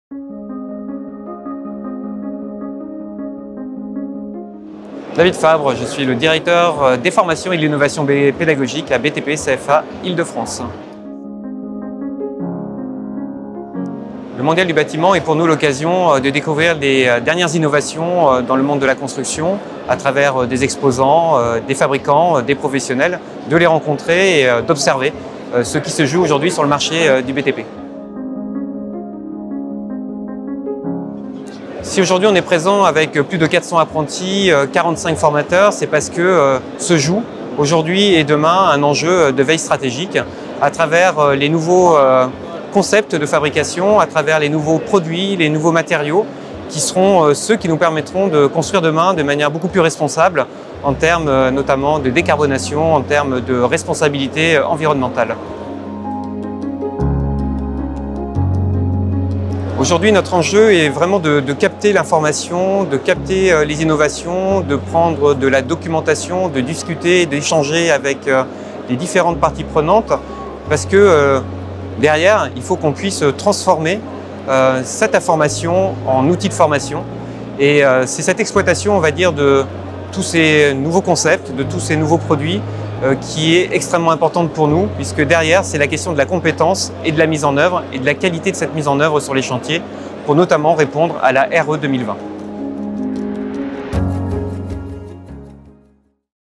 David Fabre, Directeur des formations et de l'innovation pédagogique. Intervention sur BATIMAT 2022
Le 05 octobre 2022, 400 apprentis et 45 formateurs de BTP CFA Ile-de-France se sont donné rendez-vous au salon professionnel de la construction à Paris Expo Porte de Versailles pour découvrir l’avenir du BTP.
Depuis 60 ans, Batimat connecte les pros à l’innovation. Tous les professionnels du bâtiment et de l’architecture s’y rencontrent pour échanger et découvrir des produits et des solutions innovantes dans le monde de la construction.